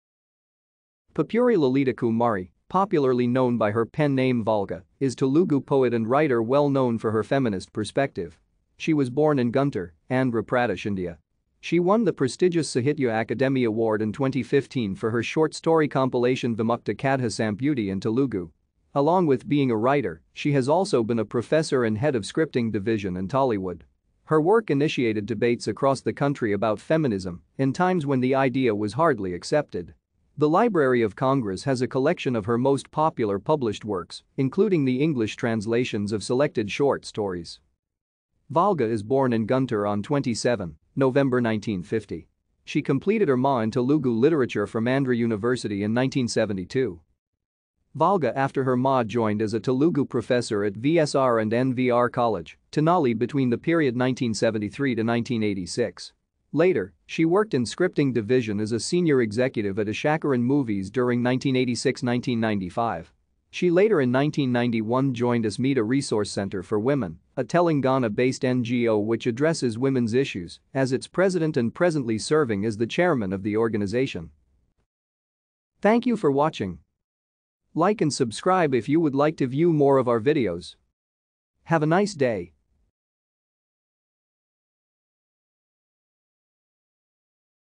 P. Lalita Kumari
Popuri Lalita Kumari, popularly known by her pen name Volga, is Telugu poet and writer well known for her feminist perspective. She was born in Guntur, Andhra Pradesh, India. She won the prestigious Sahitya Akademi Award in 2015 for her short story compilation 'Vimukta Kadha Samputi' in Telugu. Along with being a writer, she has also been a professor and head of scripting division in Tollywood. Her work initiated debates across the country about feminism, in times when the idea was hardly accepted. The Library of Congress has a collection of her most popular published works, including the English translations of selected short stories.